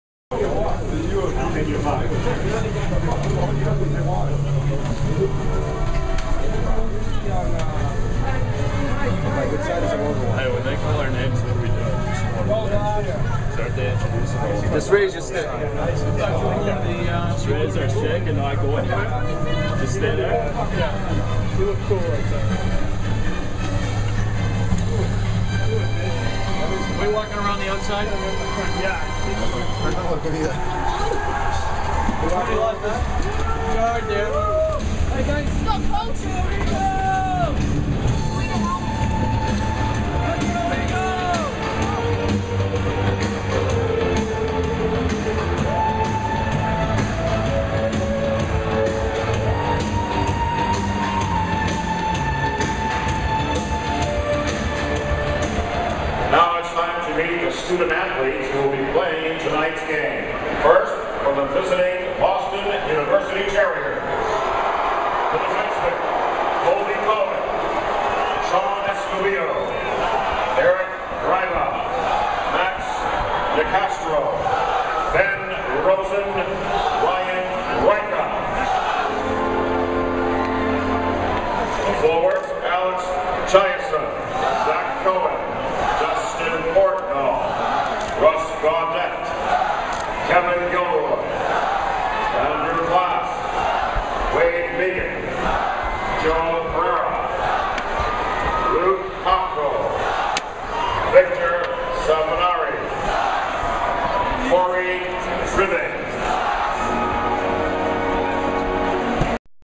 Terriers Enter Frozen Fenway
Video of the Boston University men's hockey team as it enters the field for the Frozen Fenway contest against Boston College on Jan. 8, 2010.